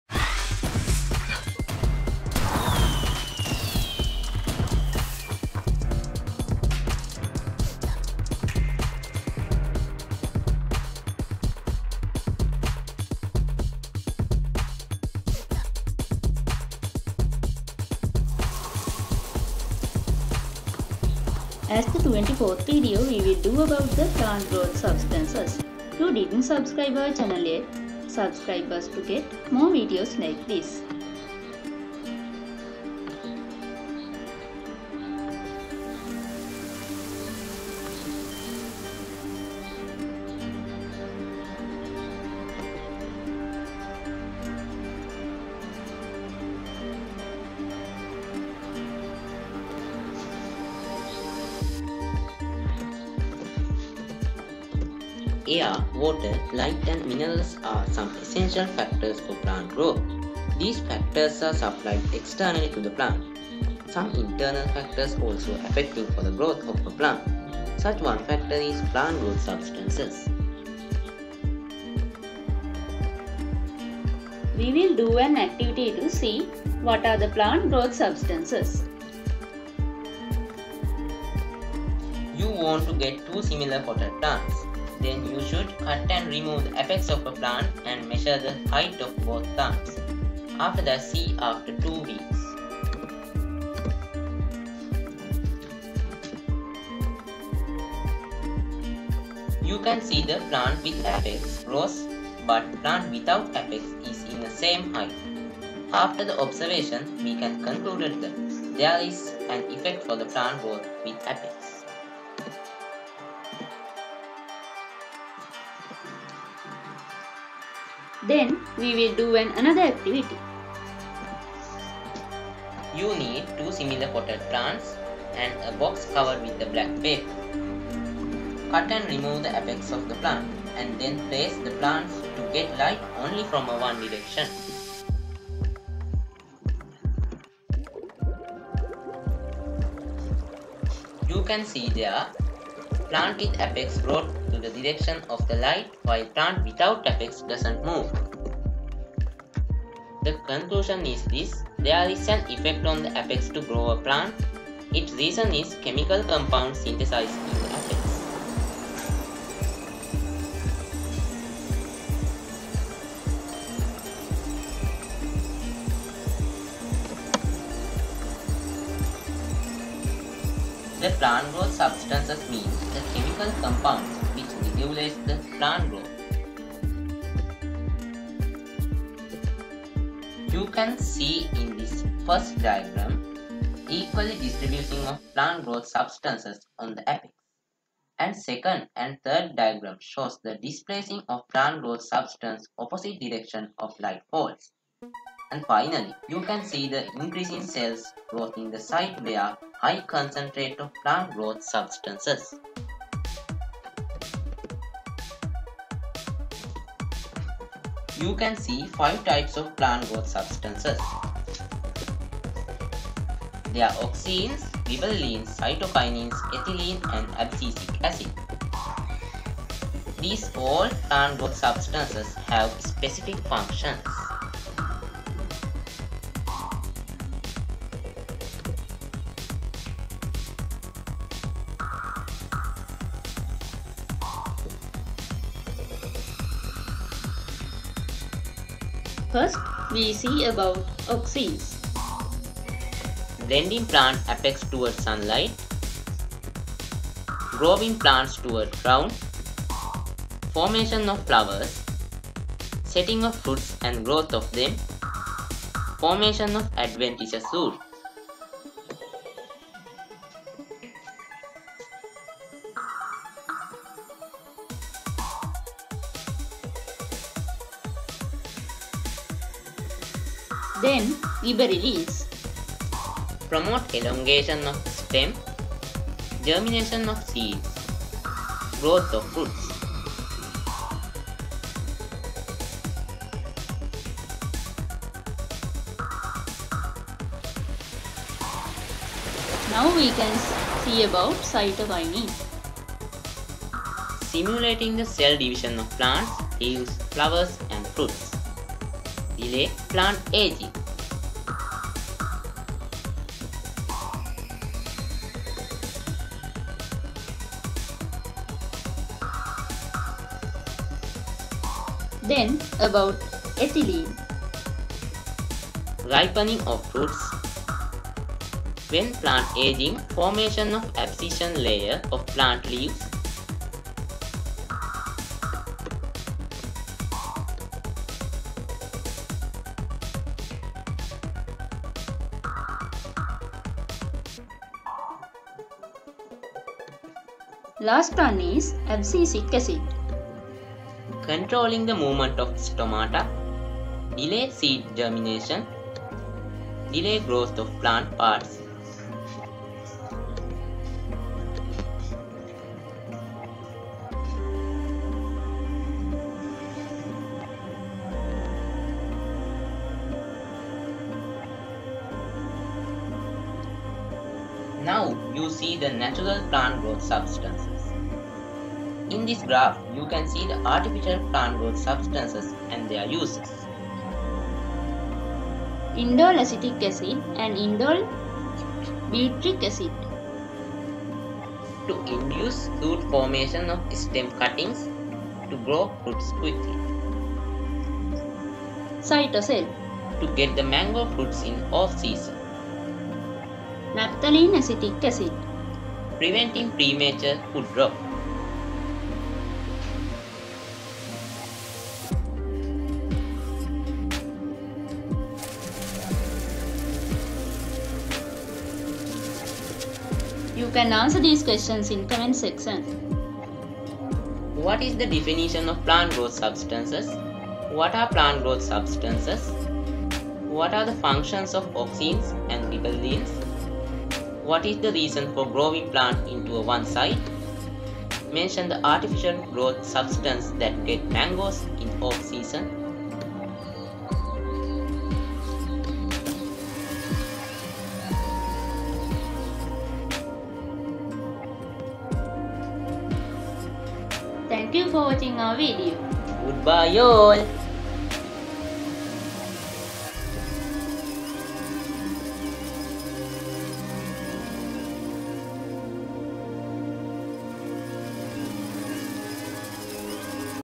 Plant growth substances| Grade 9 | Science | Biology | " VS BROSIS " | English Medium | Kid Science
This video is made with all the syllabus of Plant growth substances Auxins, Gibberilines, etc
You can see the above link. It has all the videos of Science on our channel. You can provide suggestions also for our channel. Pl. Remember see our channel.🙏👍✋.